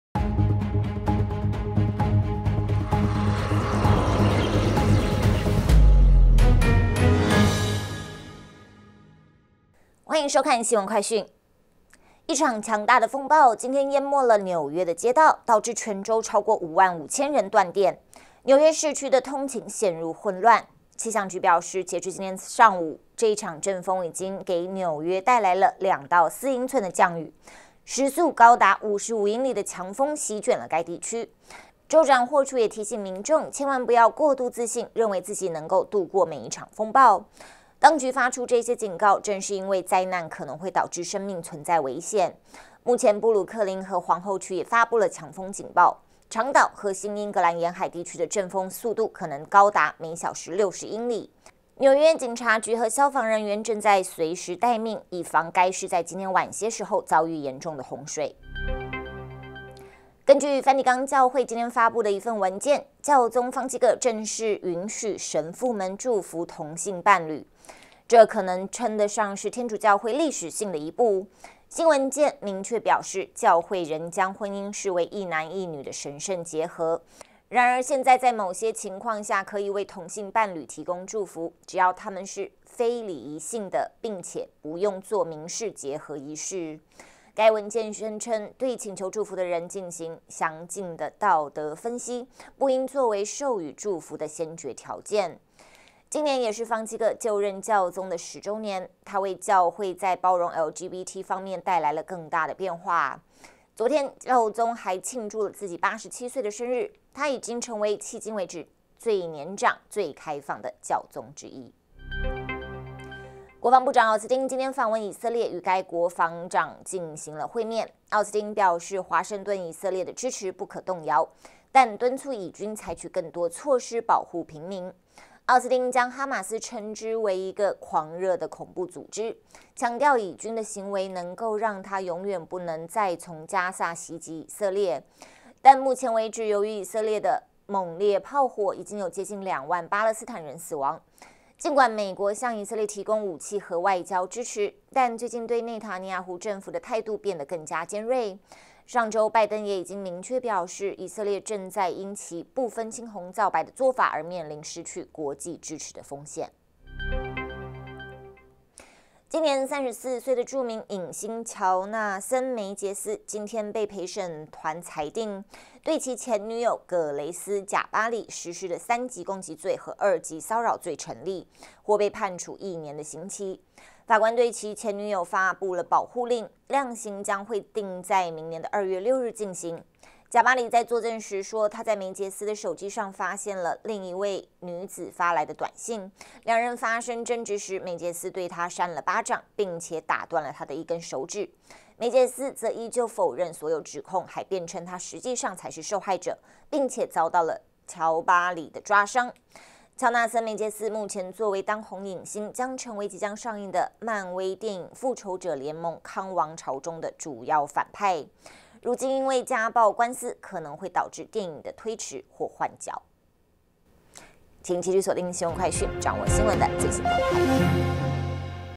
新聞快訊 | 教宗方濟各批准祝福同性伴侶
紐約遇風暴 帶來洪水和大規模停電

今天，一場強大的風暴淹沒了紐約的街道，導致全州超過5萬5千人斷電，紐約市區的通勤陷入混亂。氣象局表示，截至今天上午，這場陣風已經給紐約帶來了兩到四英寸的降雨，時速高達55英里的強風席捲了該地區。州長凱西·霍楚也提醒民眾，前往不要過度自信，認為自己能夠渡過每一場風暴，當局發出這些警告，正是因為災難可能會導致生命存在危險。目前，布魯克林和皇后區也發布了「強風警報」，長島和新英格蘭沿海地區的陣風速度可能高達每小時60英里。紐約警察局和消防人員正在隨時待命，以防該市在今天晚些時候遭遇嚴重的沿海洪水。

教宗方濟各批准祝福同性伴侶

根據梵蒂岡教會今天發布的一份文件，教宗方濟各正式允許神父們祝福同性伴侶，這可以稱得上是天主教會歷史性的一步。新文件明確表示，教會仍將婚姻視為一男一女的神聖結合。然而，現在在某些情況下可以為同性伴侶提供祝福，只要它們是非禮儀性的並且不用作民事結合儀式。該文件宣稱，對請求祝福的人進行「詳盡的道德分析」不應作為「授予祝福的先決條件」。今年也是方濟各就任教宗的十週年，他為教會在包容LGBT方面帶來了更大的變化。昨天，教宗還慶祝了自己87歲的生日，他已成為迄今為止最年長、最開放的教宗之一。

國防部長訪問以色列 呼籲保護加薩平民

國防部長勞埃德·奧斯汀今天訪問以色列，與該國防長進行了會面。奧斯汀表示，華盛頓對以色列的支持「不可動搖」，但敦促以軍採取更多措施保護平民。奧斯汀將哈馬斯稱之為一個「狂熱的恐怖組織」，強調以軍的行為能夠讓它永遠不能再從加薩襲擊以色列。但目前為止，由於以色列的猛烈炮火，已經有接近2萬巴勒斯坦人死亡。儘管美國向以色列提供武器和外交支持，但最近對內塔尼亞胡政府的態度變得更加尖銳。上週，拜登也已經明確表示，以色列正在因其「不分青紅皂白」的做法而面臨失去國際支持的風險。

漫威影星梅傑斯毆打前女友罪名成立

今年34歲的著名影星喬納森·梅傑斯今天被陪審團裁定，對其前女友格蕾絲·賈巴里 (Grace Jabbari) 實施的三級攻擊罪和二級騷擾罪成立，或被判處1年刑期。法官對其前女友發佈了保護令，量刑將會定在明年2月6日進行。賈巴里在作證時說，她在梅傑斯的手機上發現了另一名女子發來的短信，兩人發生爭執時，梅傑斯對她扇了巴掌，並且打斷了她的一根手指。梅傑斯則依舊否認所有指控，還辯稱他實際上才是受害者，並且遭到了賈巴裡的抓傷。喬納森·梅傑斯目前作為當紅影星，將成為即將上映的漫威電影《復仇者聯盟：康王朝》中的主要反派，如今因為家暴官司，可能會導致電影的推遲或換角。